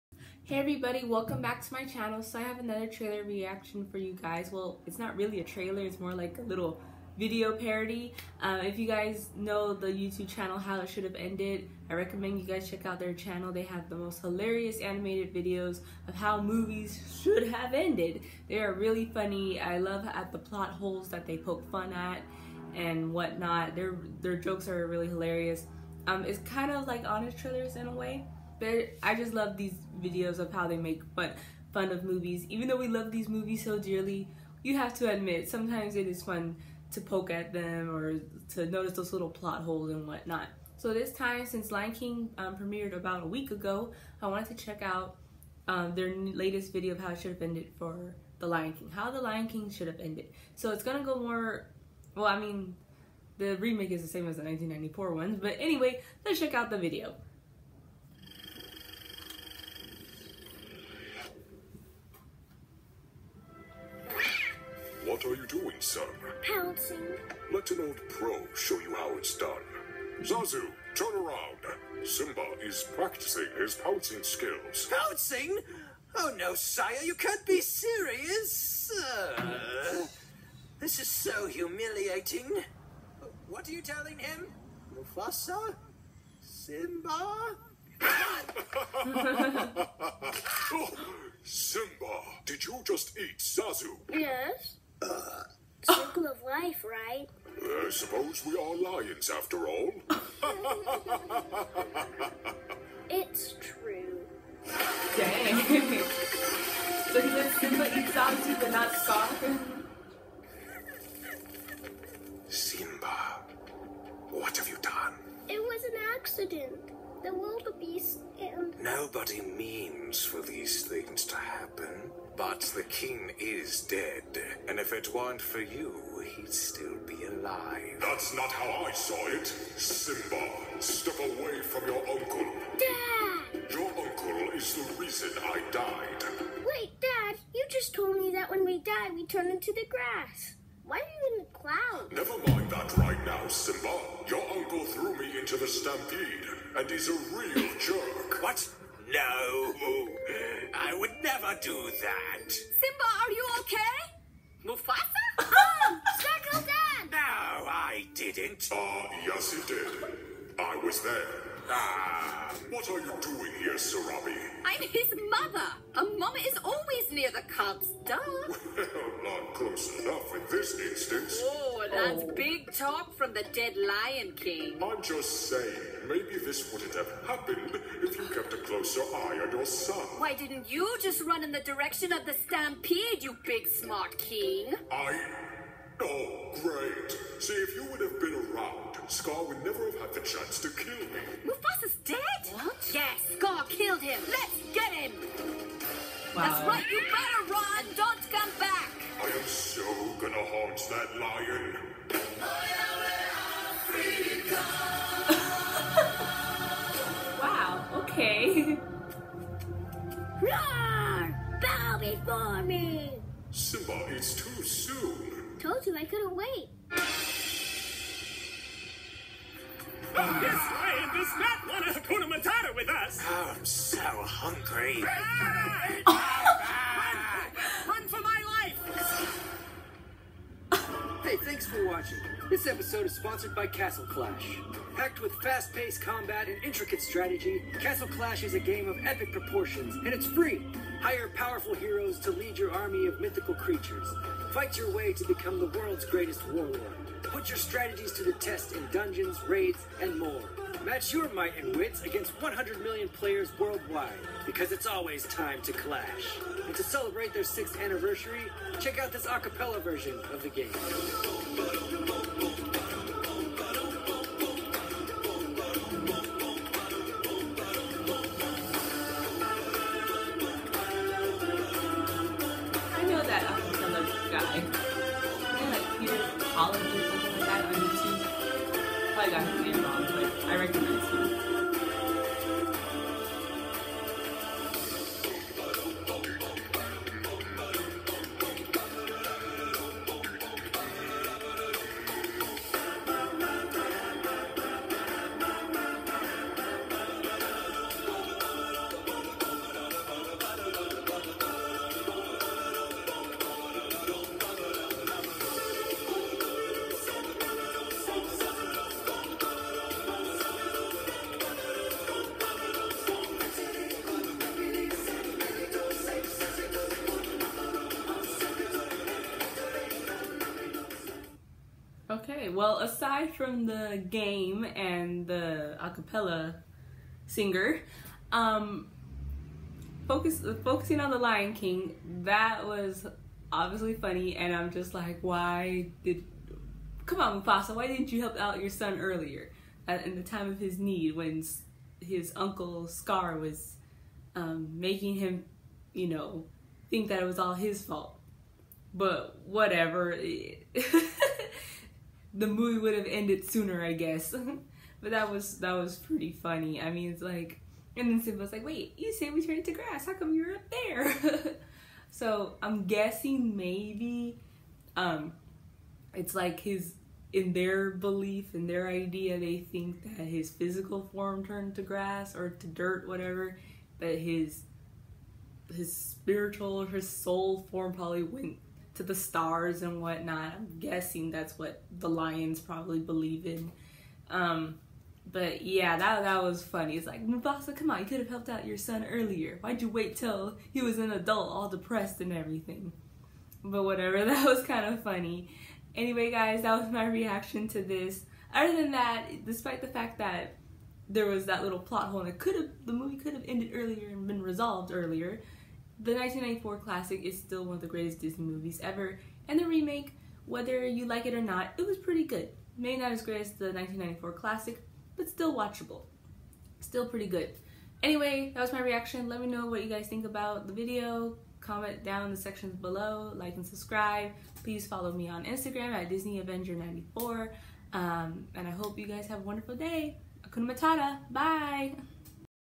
How the Lion King (1994) Should Have Ended
HISHE hilarious take on the 1994 animated Disney classic, The LionK King. 👑 🦁